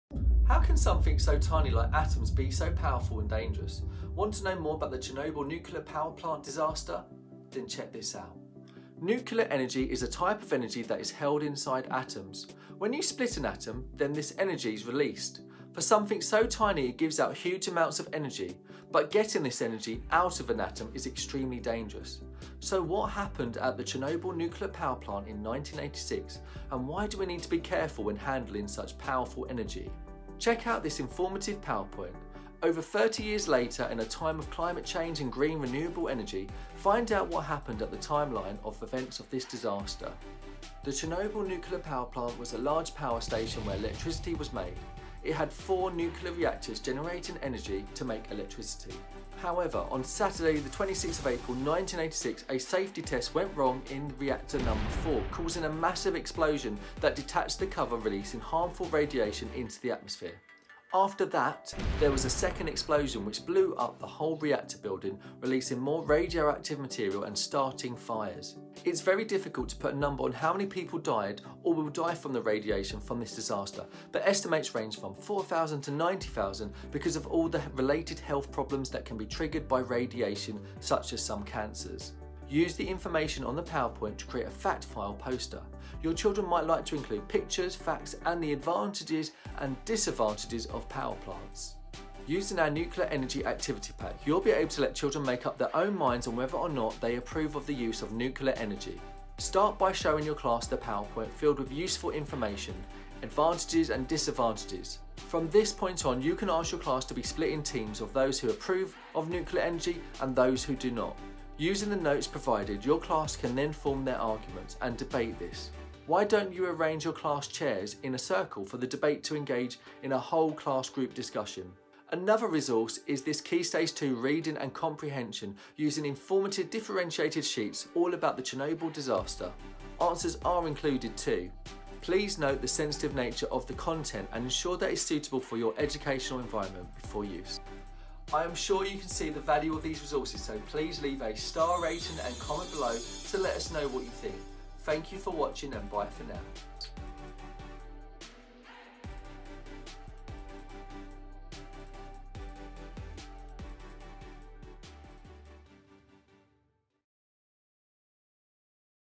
What Happened at the Chernobyl Nuclear Power Plant Disaster?
In this video, Twinkl Teacher Lloyd showcases nuclear power plant resources.   Over 30 years later, in a climate of climate change and green, renewable energy, find out what happened at the Chernobyl Nuclear Power Plant in 1986. What was the timeline of events and what happened afterwards?

0:00 Introduction
0:10 What is Nuclear Energy?
0:20  What Happened at the Chernobyl Nuclear Power Plant Disaster?
0:40 What Went Wrong?
0:50 Radiation Cloud
1:14 The Future of Nuclear Power
1:21 Fact-file Poster Design Activity
1:34 Nuclear Power Activity Resource Pack
2:05 Chernobyl Comprehension Reading Pack